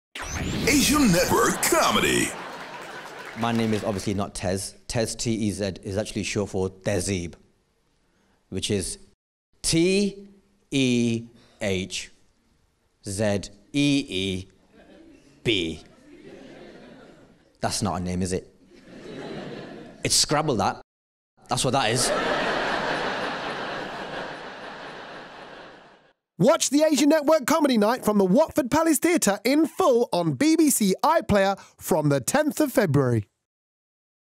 Tez Ilyas - Tez
A short animation of Tez's thoughts on his name. Recorded at the Watford comedy night.

See more from the Asian Network Comedy night at Watford Palace Theatre